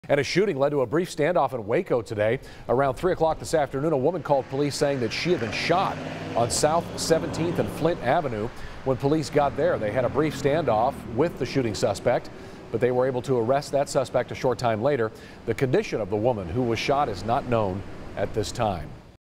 Police: Shooting suspect in custody after brief standoff in Waco, 1 injured
Around 3:15 p.m., officers received a call from a female who said she had been shot in the area of S. 17th Street and Flint Avenue, Waco Police said.